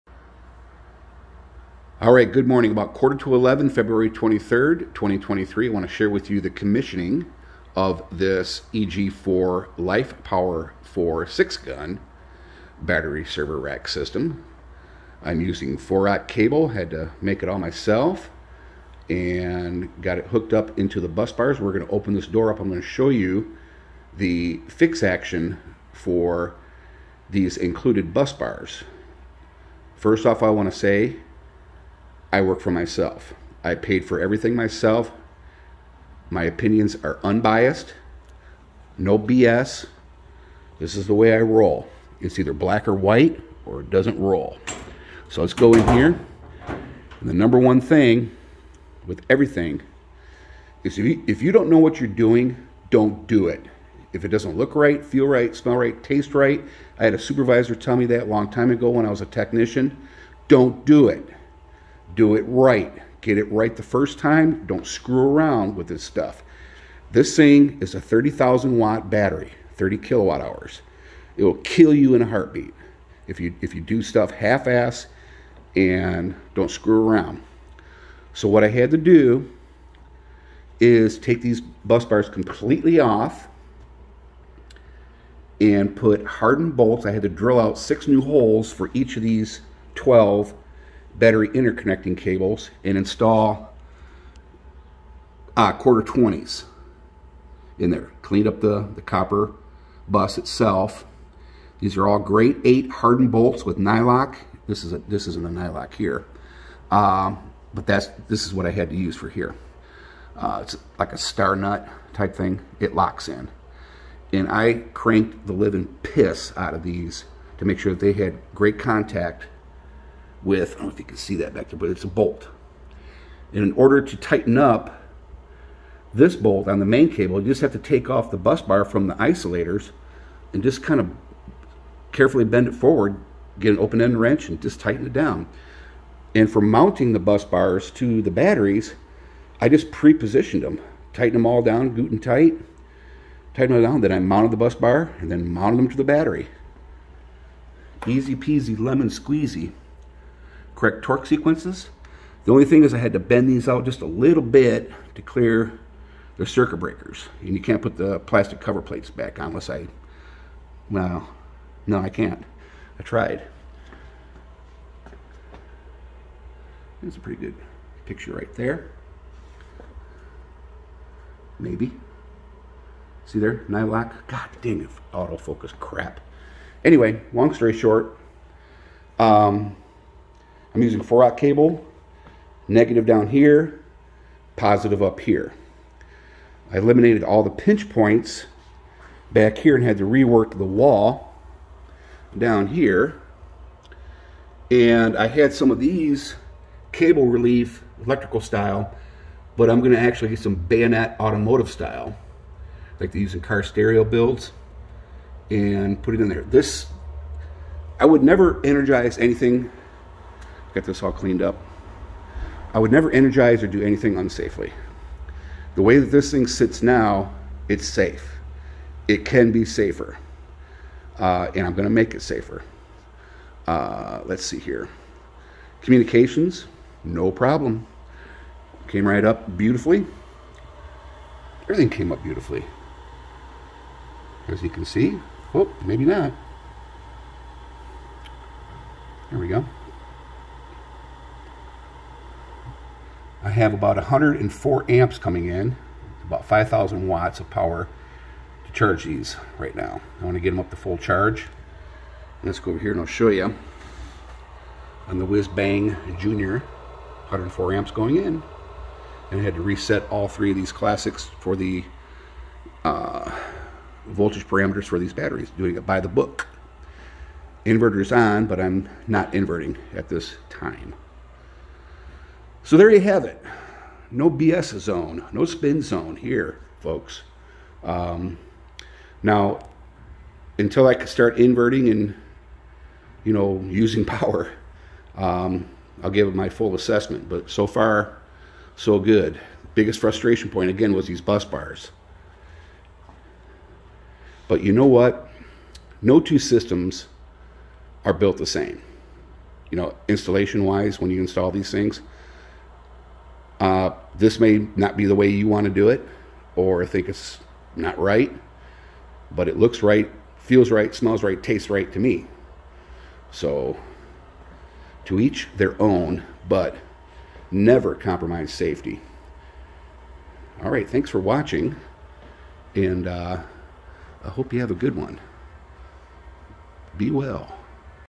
48v (30 kWh) EG4 Lifepower4 LiFePO4 battery rack system - commissioning, busbars fix action
Finally got this battery rack system online and charging. The #1 issue I had to overcome and reengineer is with the included copper busbars (with poorly designed rivnut inserts to mount wires to). In this video, I show how I DIY fixed them and attach heavy duty 4/0 welding cable runs (lugs held to the busbars with 5/16" Grade 8 bolts and lock nuts). It now has SOLID wire connections landed on the copper busbars...I torqued all the lugs down hard.

SAFETY IS #1. With 30 kWh of power and current, death on contact is possible. Think of this as a 30,000w or 600 amp arc welder. Always do things safely. If you encounter something that don't look right, it probably isn't. Trust your gut instincts, take your time, and think through the task at hand. Best practice is to consult a competent solar engineer, a licensed solar power installer, the manufacturer, and/or certified electrician experienced in this type of equipment if you have any doubts.

Chris DIYer Amazon Store: